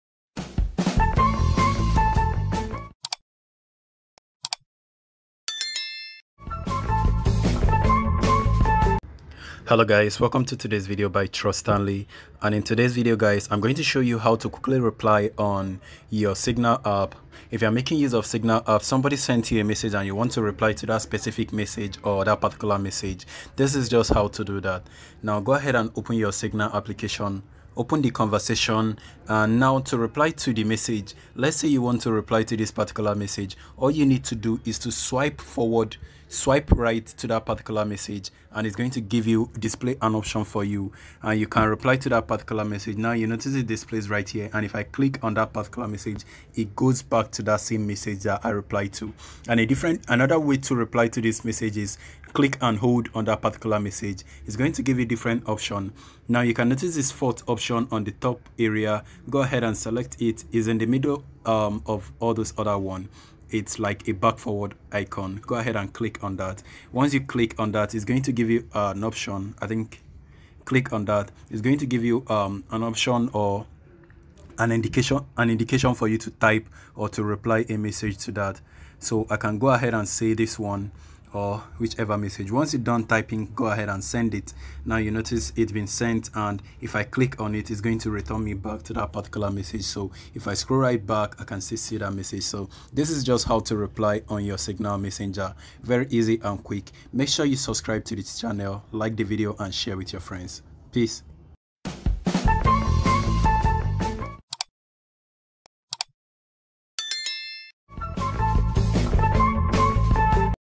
How to quick reply on Signal app | How to use Signal
Learn how to quick reply on Signal private messaging app in 2 minutes, when you send a message and decides to reply to a particular message in a Signal app chat converstaion is quiet easy to do but most people still dont know how its done so in todays video i will show you how tyo reply to a particular converstaion in signal chat.